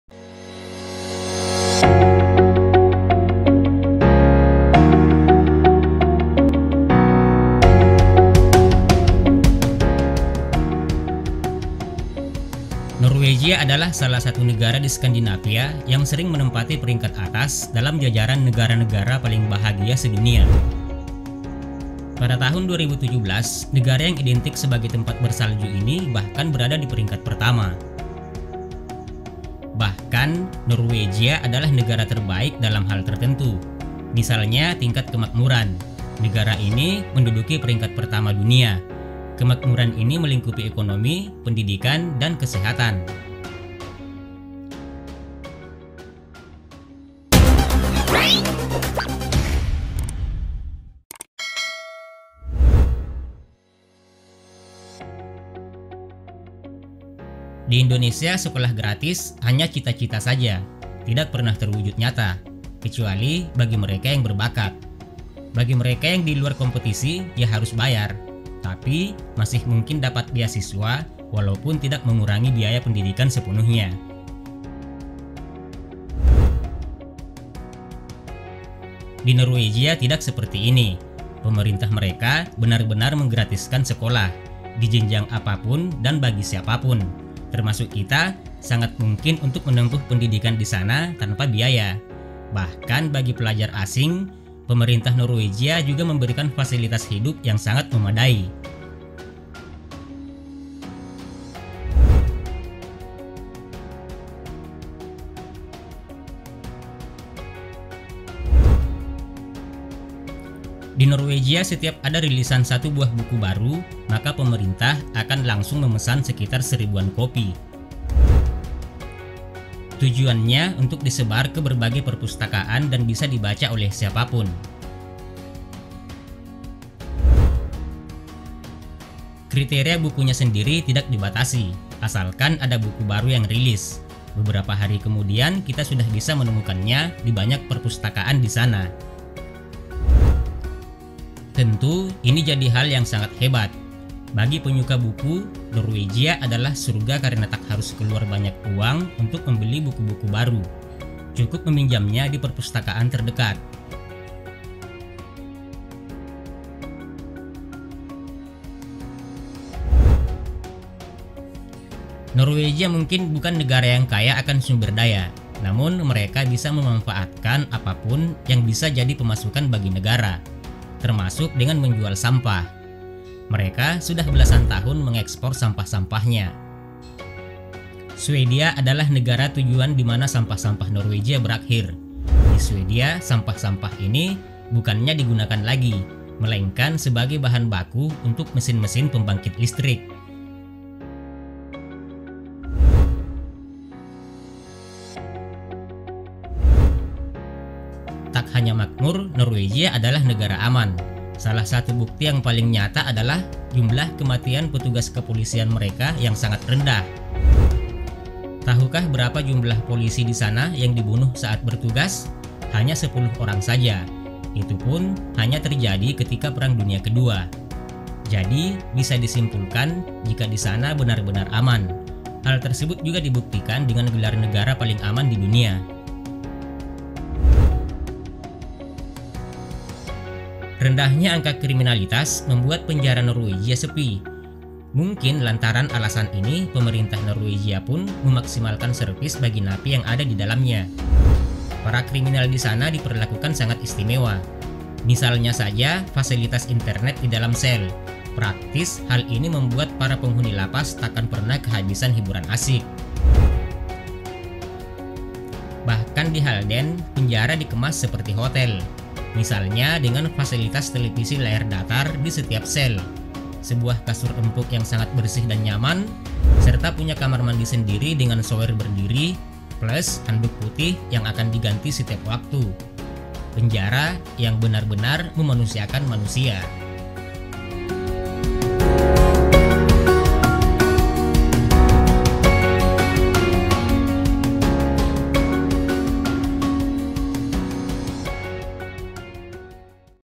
Fakta Unik Negara Paling Bahagia Sedunia | negara paling indah on the spot wisata
Norwegia adalah salah satu negara di Skandinavia yang sering menempati peringkat atas dalam jajaran negara-negara paling bahagia se-dunia. Pada tahun 2017, negara yang identik sebagai tempat bersalju ini bahkan berada di peringkat pertama.

Bahkan Norwegia adalah negara terbaik dalam hal tertentu. Misalnya tingkat kemakmuran, negara ini menduduki peringkat pertama dunia. Kemakmuran ini melingkupi ekonomi, pendidikan dan kesehatan. negara paling indah. calon sarjana nordik on the spot wisata invoice indonesia